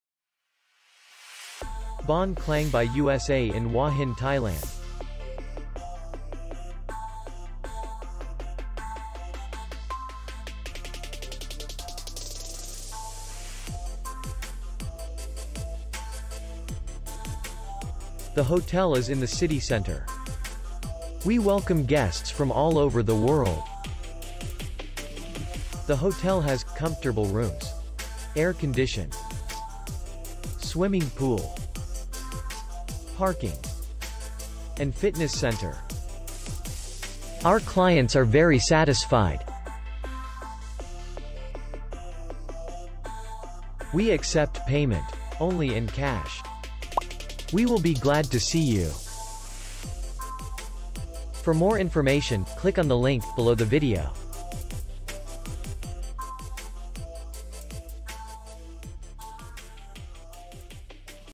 Baan Klang By USA hotel review | Hotels in Hua Hin | Thailand Hotels
Baan Klang By USA hotel video review. Hotels in Hua Hin, asian 3 stars hotels.